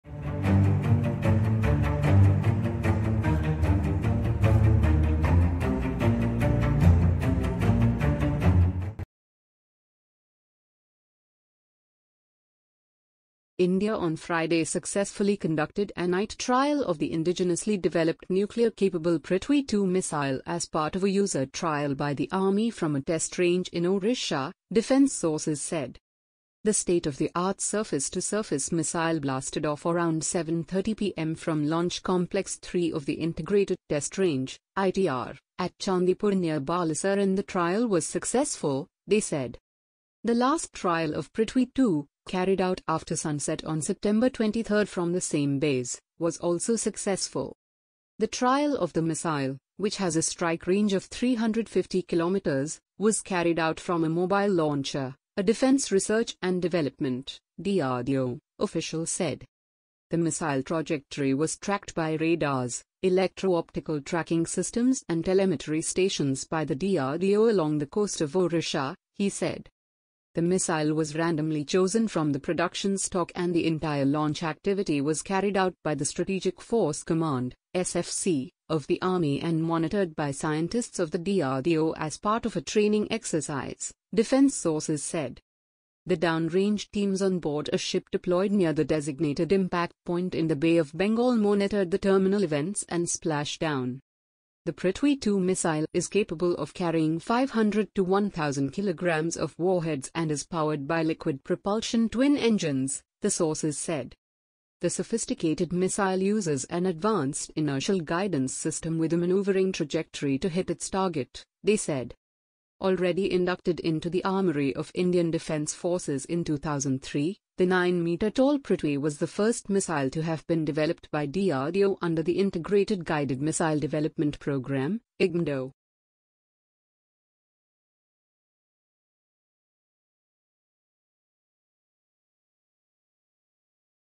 Successful night trial of nuclear-capable Prithvi-2 missile
India on Friday successfully conducted a night trial of the indigenously developed nuclear-capable Prithvi-2 missile as part of a user trial by the Army from a test range in Odisha , defence sources said.
 The state-of-the-art surface-to-surface missile blasted off around 7.30 pm from launch complex 3 of the Integrated Test Range (ITR) at Chandipur near Balasore and the trial was successful, they said.
 The last trial of Prithvi-2, carried out after sunset on September 23 from the same base, was also successful.
 "The missile trajectory was tracked by radars, electro-optical tracking systems and telemetry stations by the DRDO along the coast of Odisha," he said.
 The Prithvi-2 missile is capable of carrying 500-1,000 kg of warheads and is powered by liquid propulsion twin engines, the sources said.